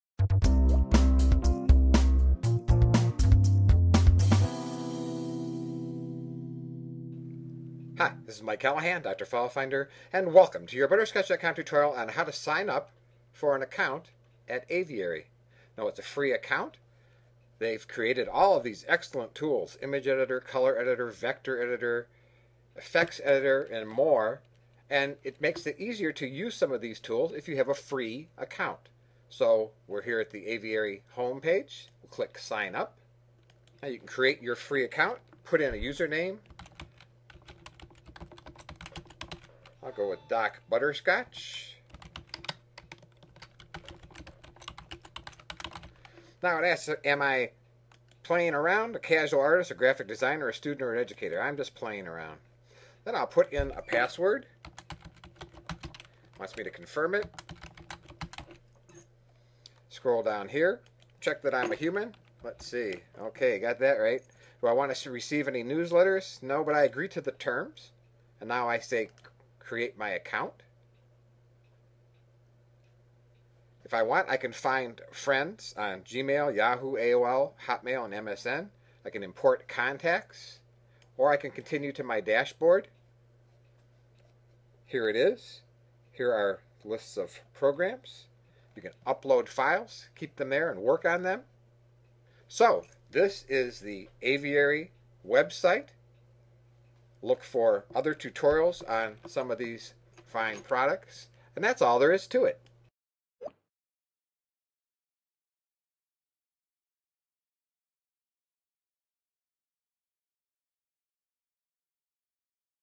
How to create an Aviary account
Aviary Inc. has created a series of excellent tools including Image Editor, Effects Editor, Image Markup among others. A Internet tutorial by butterscotch.com.